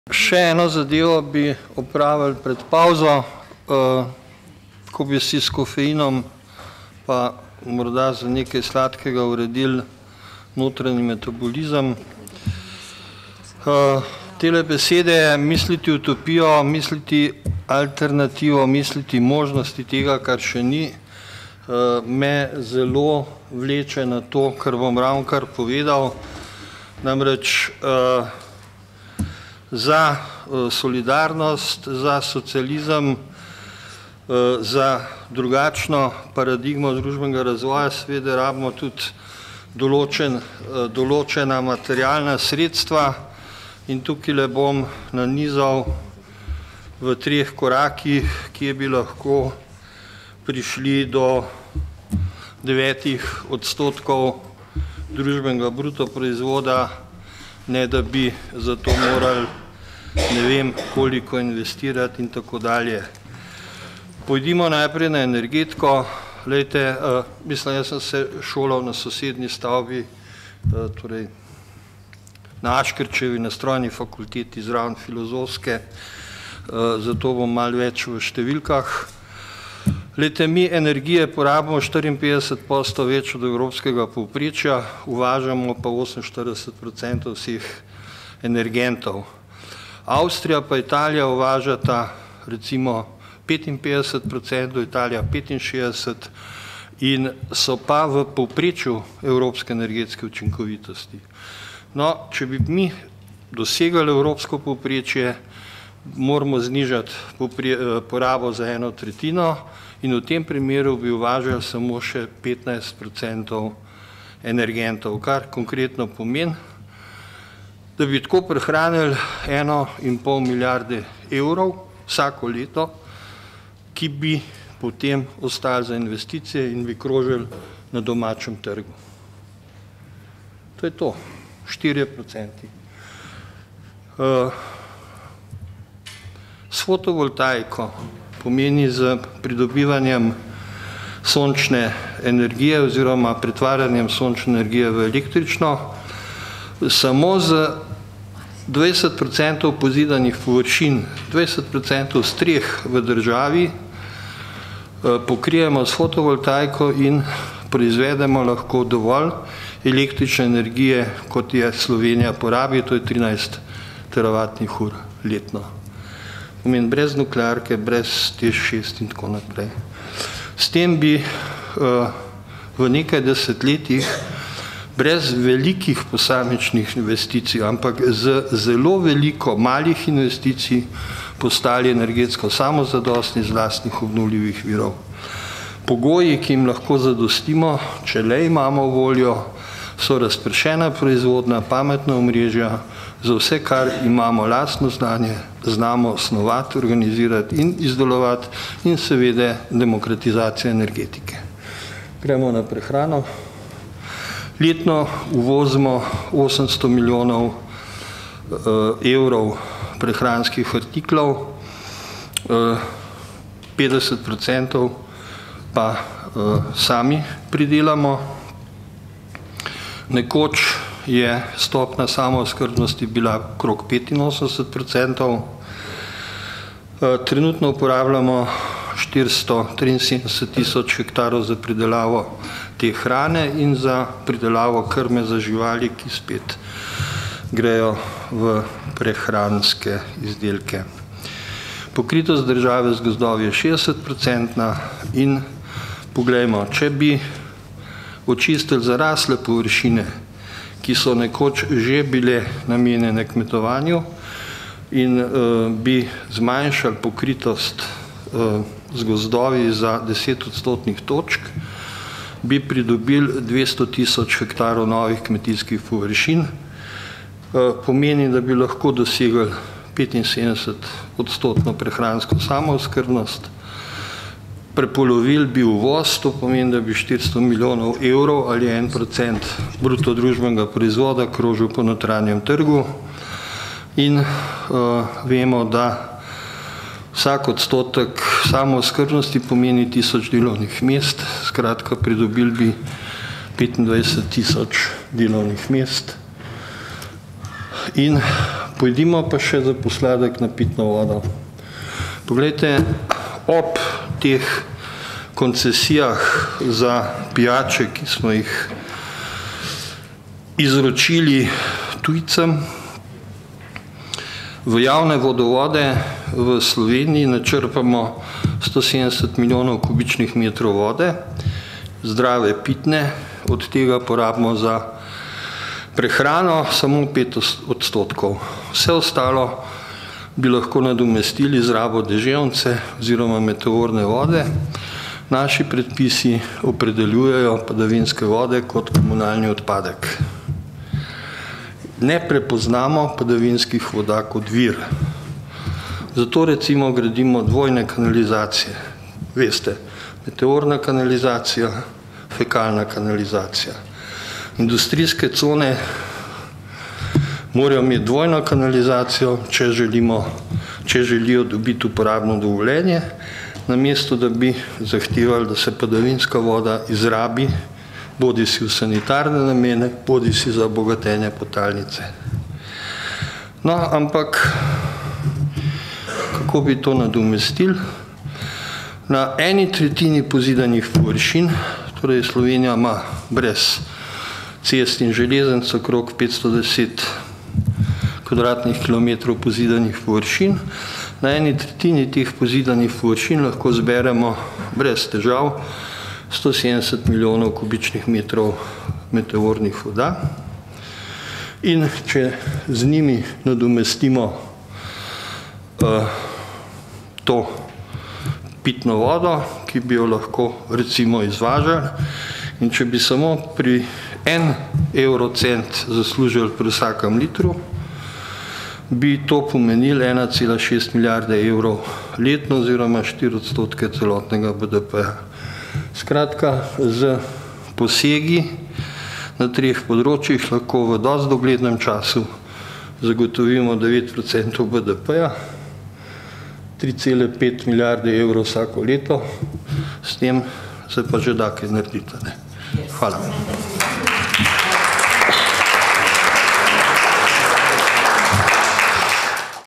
Gorazd Marinček na posvetu 21. stoletje in socialistične alternative
Gorazd Marinček na posvetu 21. stoletje in socialistične alternative, ki je potekal 24. maja 2016 v dvorani Državnega sveta v Ljubljani, predaval na temo: Energetska in prehrambna samooskrba kot gospodarski pogoj za ekosocializem.